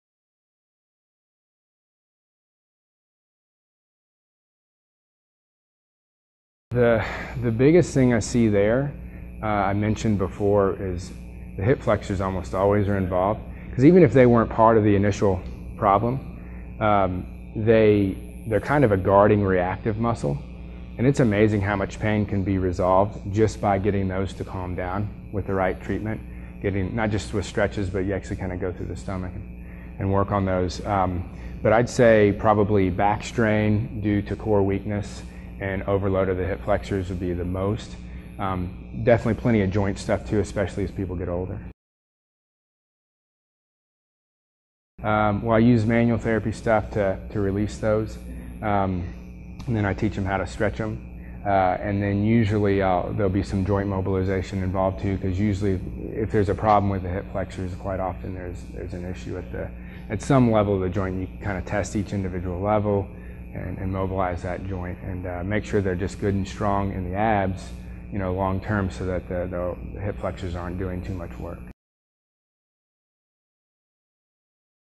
"What are the most common issues in your patients with back pain?"
Dr Jarod Carter DPT answers the questions: "What are the most common issues you find in your patients with back pain?", and "How do you treat hip flexor problems?" This was part of the Q&A session after teaching "Injury Prevention Screening" to a group of personal trainers.